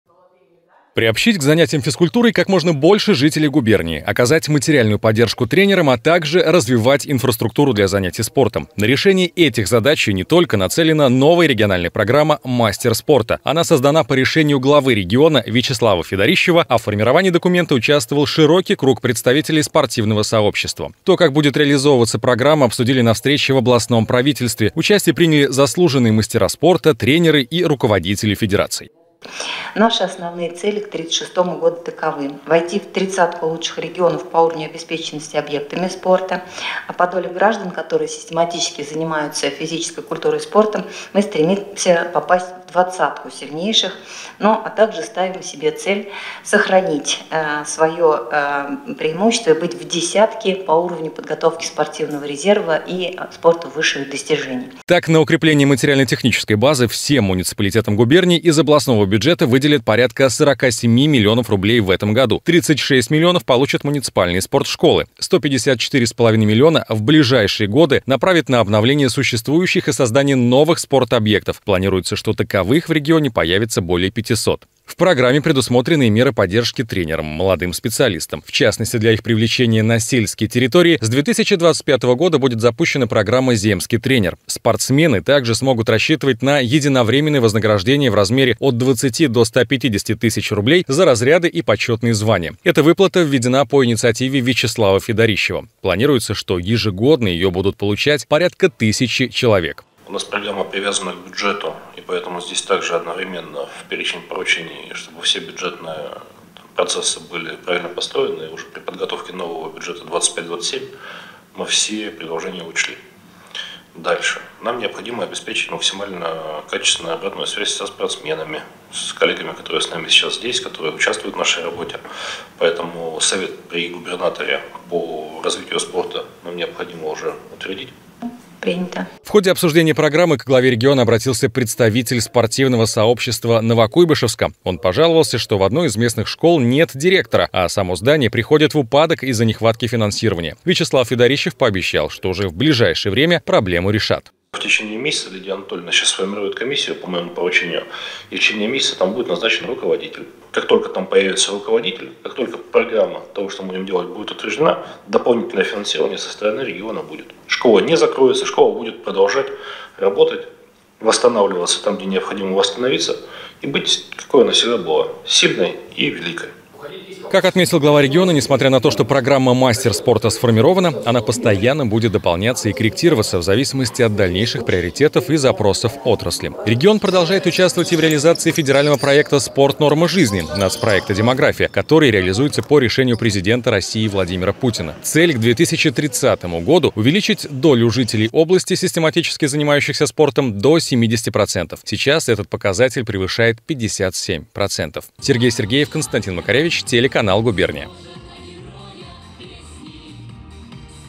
Старт региональной программы "Мастер спорта" в Самарской области
Приобщить к занятиям физкультурой как можно больше людей, оказать материальную поддержку тренерам, а также развивать соответствующую инфраструктуру. На решение этих задач и не только нацелена новая региональная программа "Мастер спорта". Она создана по решению главы Самарской области Вячеслава Федорищева. Формировалась на основе предложений, идей и инициатив спортсменов и жителей губернии.